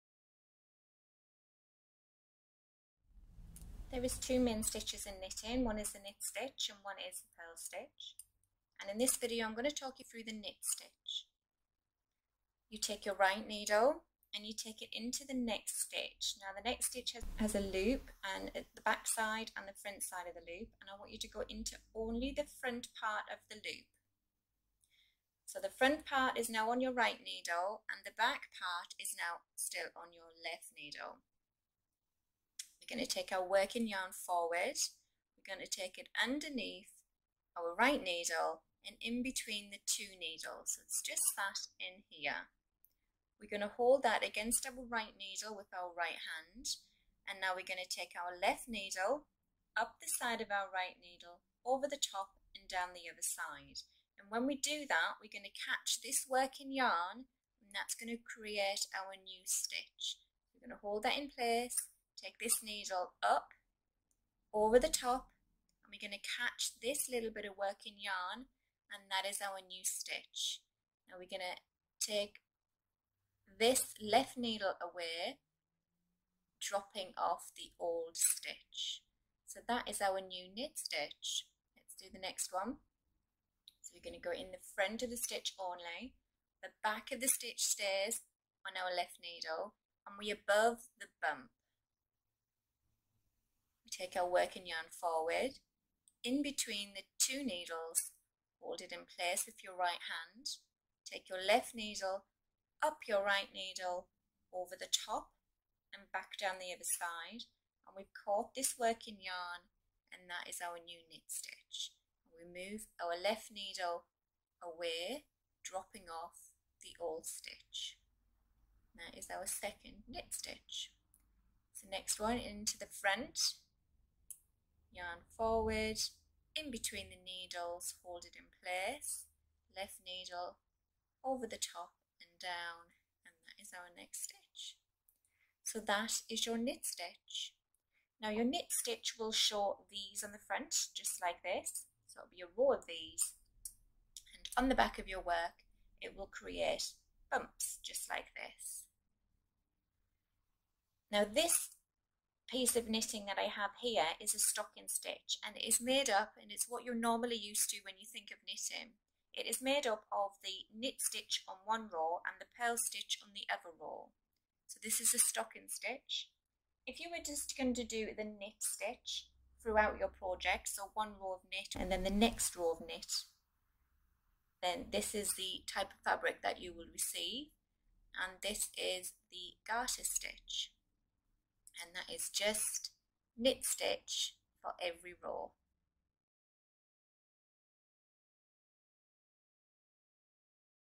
Knit Stitch - Knitting Stitches Video #2
Learn to Knit with this video.
Slow motion of the knit stitch.
The knit stitch is used in all knitting projects and is one of the 2 main stitches.